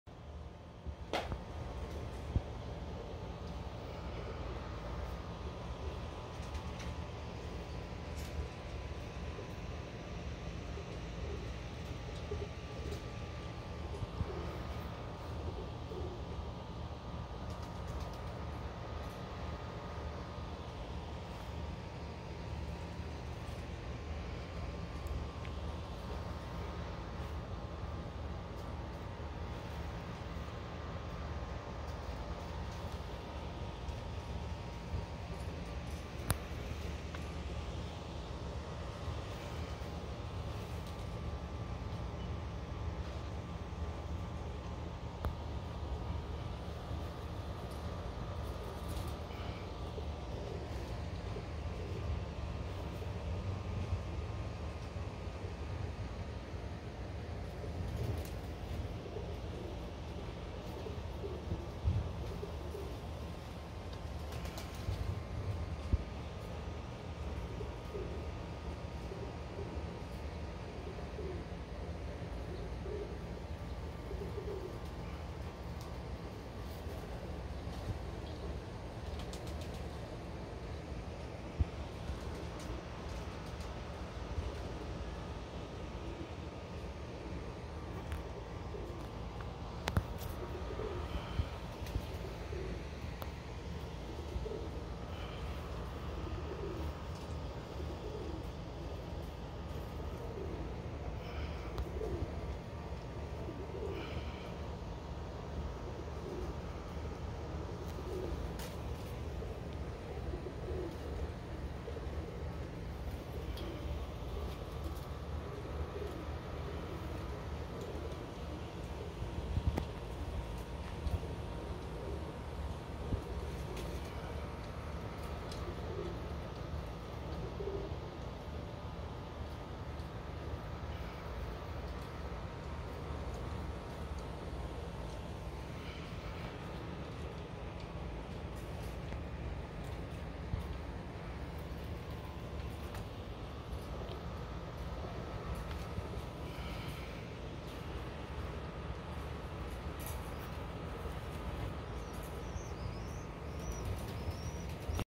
Roller pigeons 2020
Young rollers, still need more time to get deeper. One Family kit.